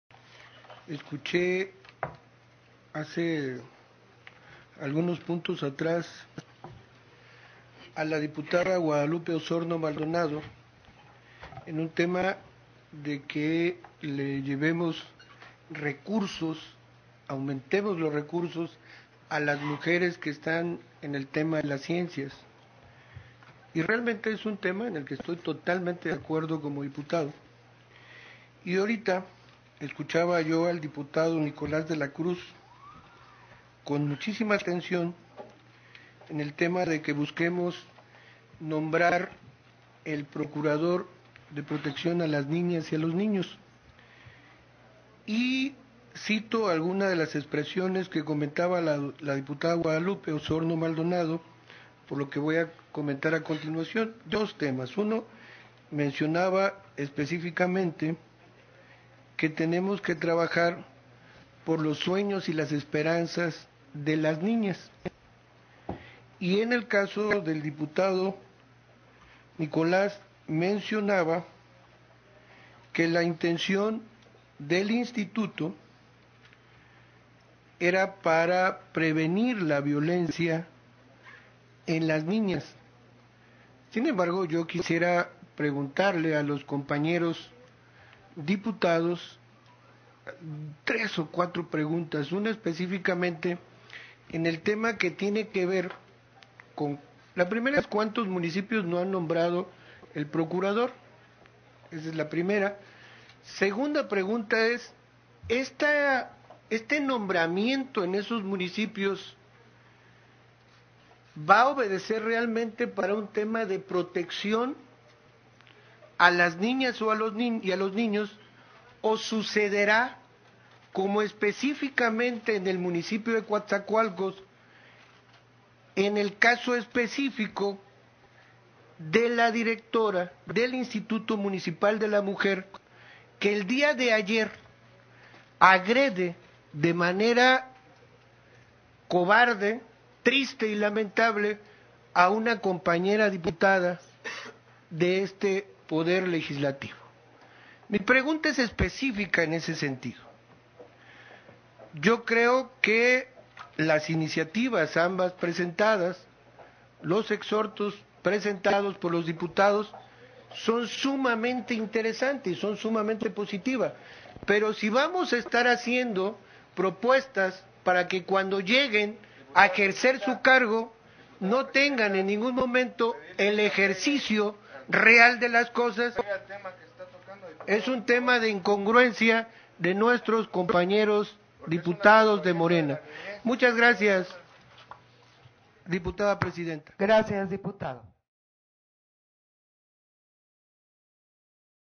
EN TRIBUNA | Diputado Tito Delfín, primera sesión ordinaria.
El diputado de Tito Delfín hace uso de tribuna